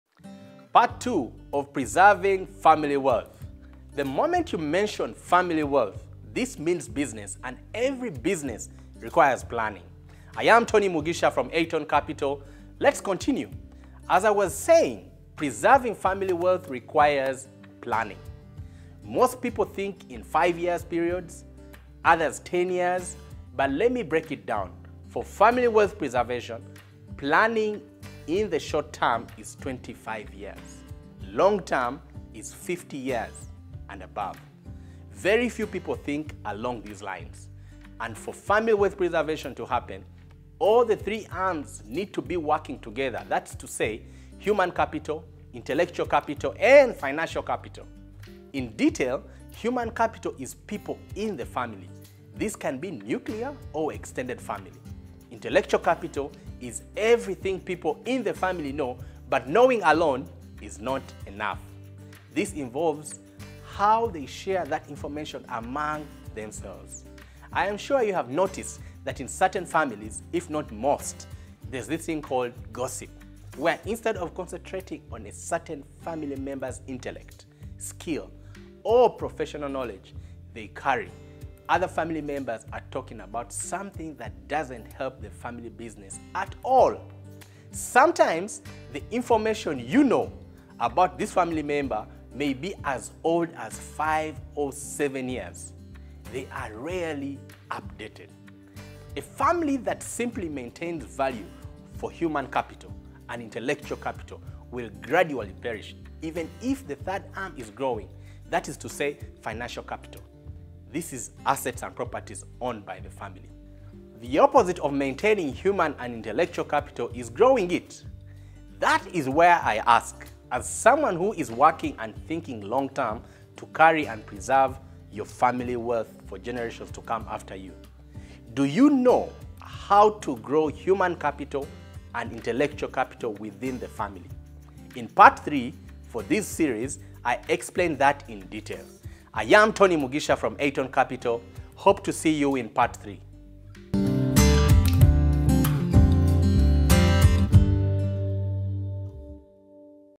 Part 2. How to Preserve Family Wealth: 5 Pillars of Family Wealth Preservation
In part 1, you'll learn:
1. The 2 main reasons why families preserve wealth
2. The difference between human capital, intellectual capital, and financial capital
3. How to assess the human capital and intellectual capital of your family

In part 2, Tony will go into more detail on each of the 5 pillars of family wealth preservation.

If you're serious about preserving your family wealth, then this video series is for you.